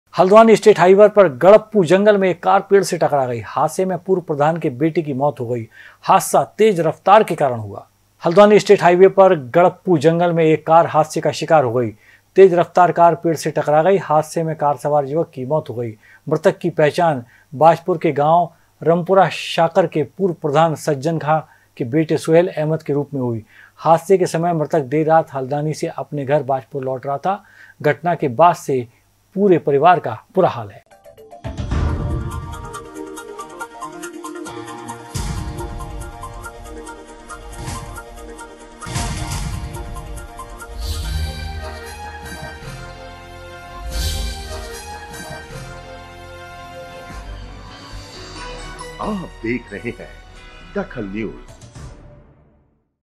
DAKHAL NEWS हाइवे किनारे पेड़ से टकराई कार
हाइवे किनारे पेड़ से टकराई कार
हल्द्वानी से बाजपुर लौटते समय हादसा